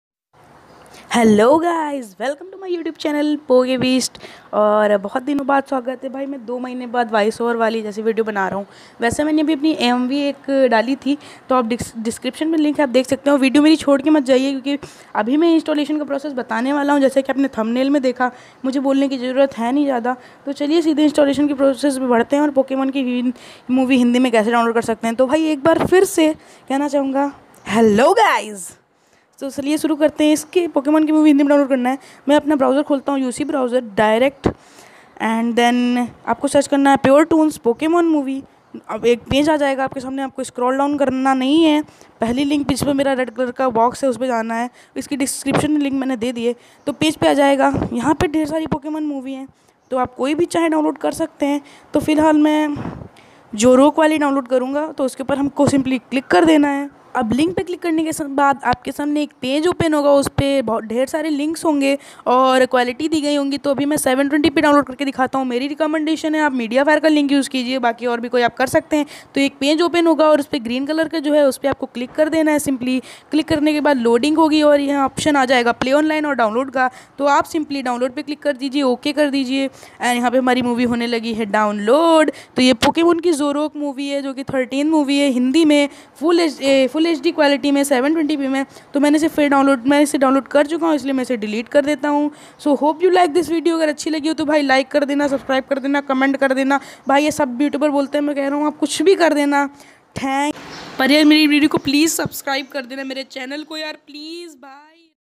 How to download All Pokemon Movie in Hindi - FHD+ Quality | Link in Discription
Hello Guys,

Pokemon Movies of all series Must Watch    👇

                Or
You Can Search -
Puretoons Pokemon movie in hindi
●And Then Click On 1st Link●

Isse baat pe thoko like👍

I Suggest you that please use MediaFire or Direct Download Link

■These link are easy to download movie
For Full Information Please Watch Full Vedio. ■

ठोंको लाइक